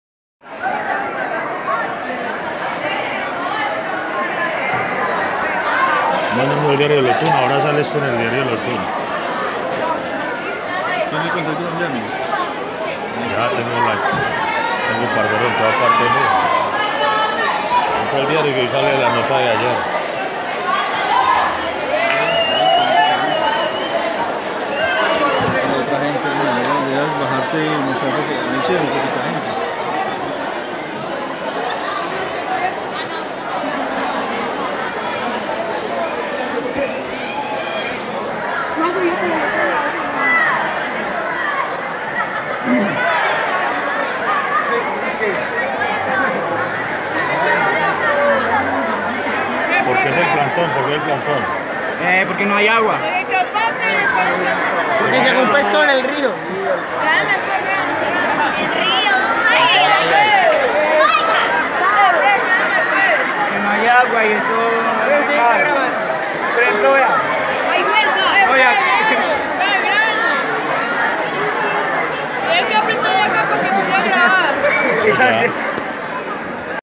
“El agua se va por arte de magia” en el colegio Liceo Gabriela Mistral de La Virginia
Cansados por la falta de suministro, los alumnos de la institución realizaron un plantón, pero la situación sigue sin resolverse.